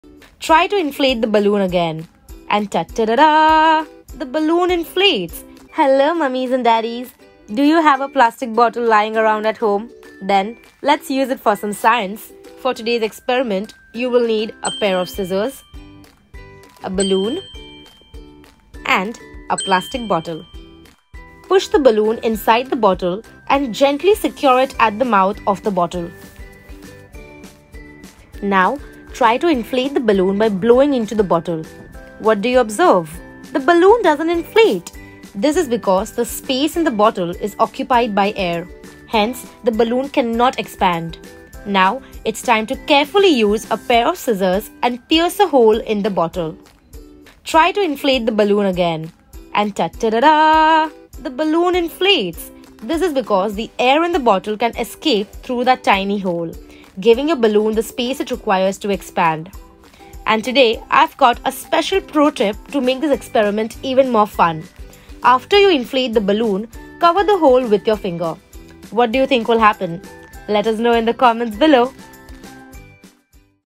Easy Activity for Children - Try at home
🎈✨ Unveiling the Science of Air: The 'Balloon in a Bottle' Experiment! 🧪🔍

Prepare to embark on a captivating journey into the world of air pressure and physics as we unveil the magic of the 'Balloon in a Bottle' experiment! Through this hands-on exploration, we'll discover the profound impact air pressure has on the inflation of a balloon and how a tiny hole can make all the difference.

The Experiment in Action:

1️⃣ Setting the Stage: To conduct this experiment, you'll need a plastic bottle, a balloon, and a keen sense of curiosity.

2️⃣ Initial Attempt: Begin by attempting to blow up the balloon while it's securely placed inside the bottle. You'll quickly notice that no matter how hard you try, the balloon refuses to inflate. It's a curious phenomenon that piques our scientific interest.

3️⃣ Introducing the Hole: Now, the magic touch! With a pin or sharp object, carefully create a small hole in the side of the bottle, ensuring it's close to the balloon without touching it.

4️⃣ The Astonishing Result: Take a deep breath and blow into the bottle once more. This time, something incredible happens—the balloon inflates! It's a captivating transformation that demonstrates the fundamental concept that air takes up space.

Unveiling the Science:

This experiment beautifully illustrates the principle of air pressure. Initially, the bottle's sealed environment prevents the air inside from escaping, making it impossible for the balloon to inflate. However, with the introduction of the small hole, a path for air to escape is created. As you blow into the bottle, the escaping air creates a lower pressure inside, allowing the higher external air pressure to push into the balloon, causing it to inflate.

Why It Matters:

This experiment not only showcases the wonders of science but also emphasizes the importance of understanding air pressure in various real-world scenarios. From weather patterns to everyday phenomena, the concept of air pressure plays a significant role in our lives.

Try this experiment at home or in the classroom, and witness the magic of science firsthand. Share your experiences, questions, and observations in the comments below, and let's continue our journey of exploration together! 🌬️🌍